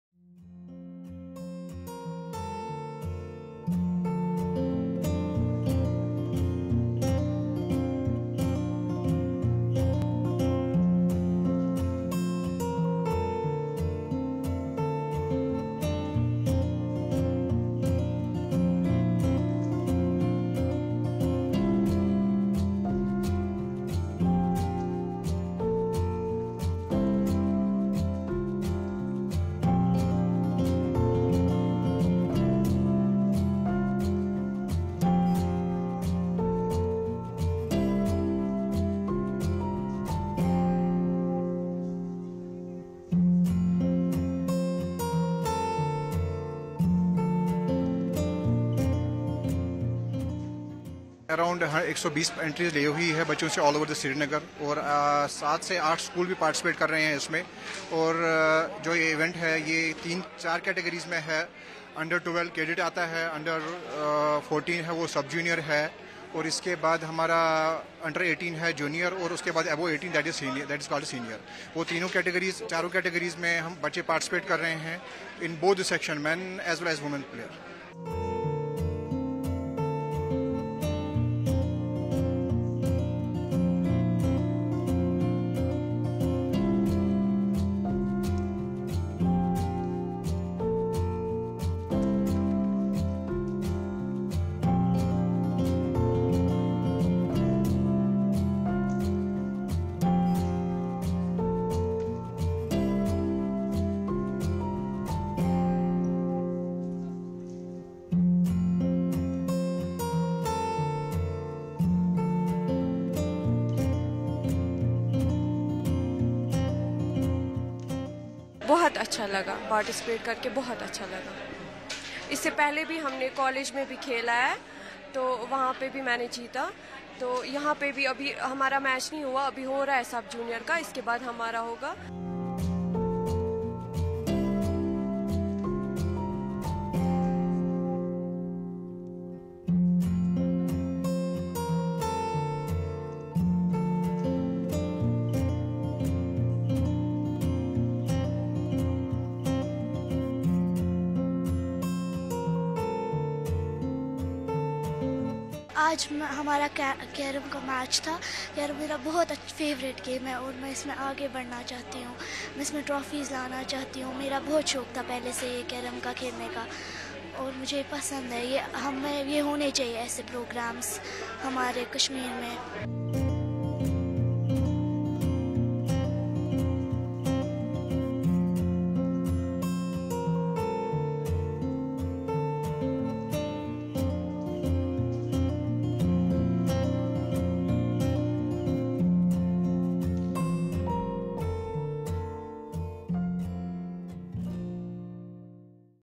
District Carrom championship organised in SrinagarI
Srinagar, August 01 (ANI): Two-day-long District Srinagar Carrom Championship was organised by District Srinagar Carrom Association at Indoor Sports Hall Polo Ground in Srinagar. It was organised in collaboration with Sports Council and District Units of Association in each and every district of the Jammu and Kashmir. The motive of Carrom Association is to boost the Carrom game in Jammu and Kashmir. It was organised for sub junior, junior and senior category for both boys and girls respectively. Around 120 participants from all the districts participated in the championship. They encouraged the players who participated in the carrom competition. Players lauded the step taken by the Carrom Association and demanded more such championships in all over the Union Territory.(TS)